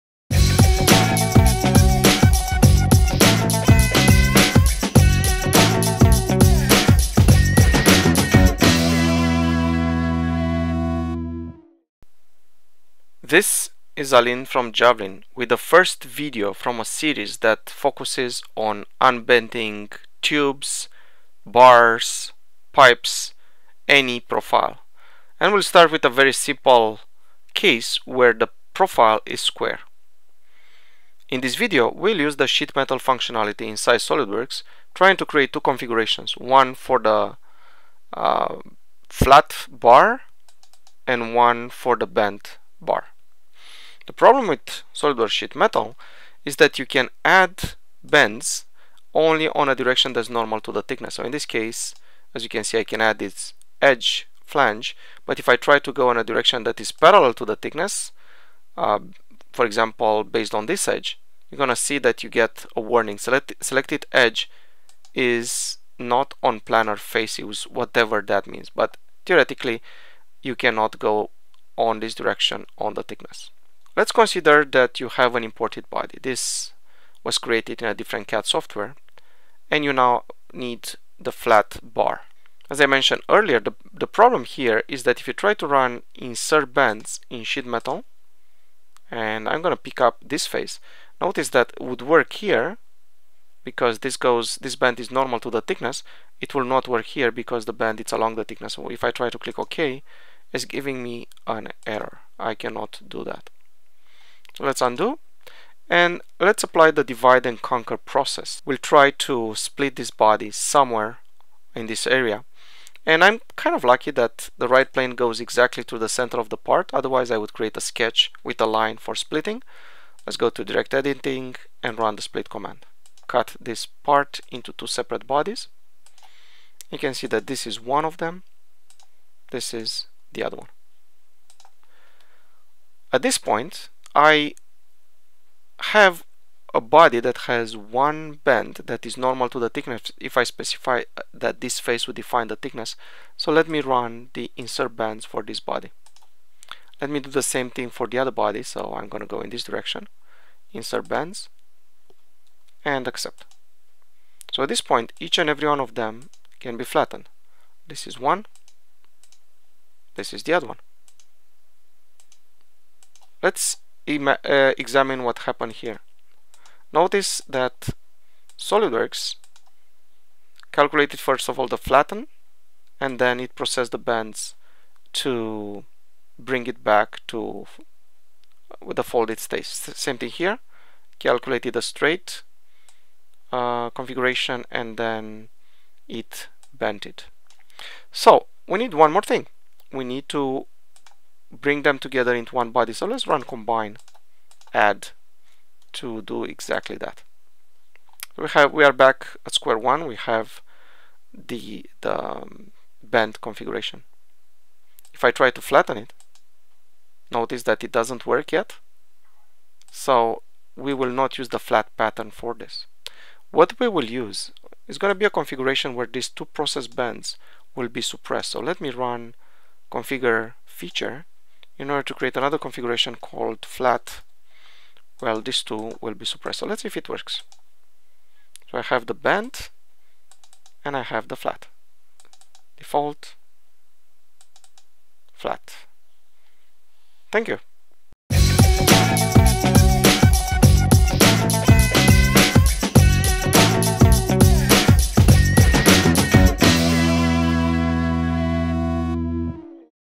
SOLIDWORKS Tutorial: Unbend a Square Bar using the Divide & Conquer Technique in Sheet Metal
Imagine you receive a bent bar as imported geometry. This video explores a simplified case, where the profile is square.

Go around the limitations in the SOLIDWORKS Sheet Metal functionality, by using the Divide and Conquer solution.